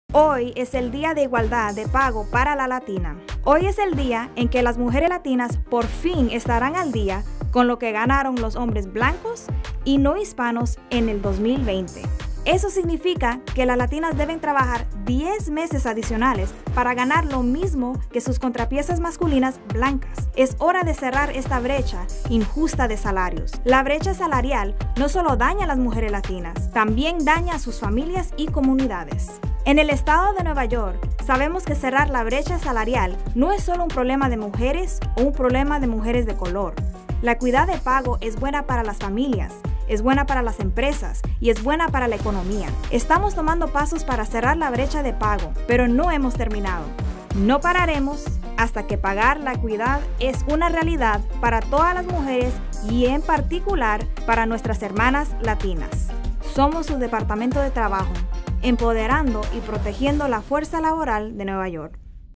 El Día de Igualdad de Pago Para la Latina
Hoy es el Día de la Igualdad Salarial de las Latinas, el día en que el salario de las Latinas alcanza a lo que ganaron los hombres blancos, no hispanos solo en 2020.